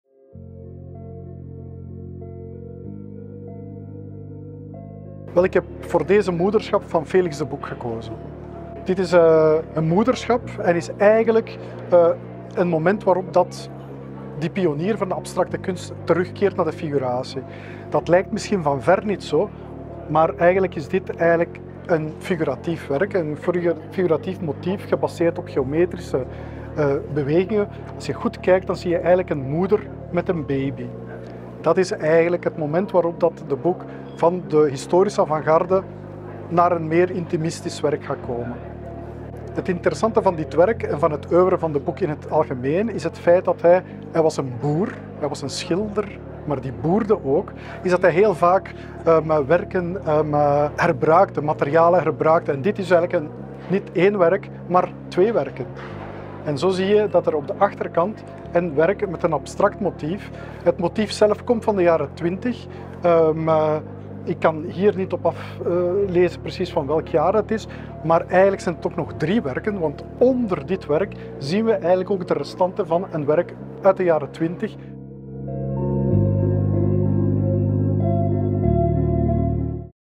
BRAFA Favs : Sergio Servellón on Felix De Boeck's Motherhood painting
The BRAFA Art Fair attracts museum directors, curators and collectors alike. FeliXart Museum director Sergio Servellón is always on the  lookout to find works by Felix De Boeck, founder and namesake of the museum. He is thrilled to share his find 'Motherhood' by the artist which he spotted on the booth of MDZ Art Gallery during BRAFA 2023.

BRAFA, created in 1956, is one of the world’s oldest and most prestigious art fairs, famous for the high quality fine art, antiques, modern art, contemporary art and design it offers. for more information and visit the fair until February 5th, 2023.

Video by NOOT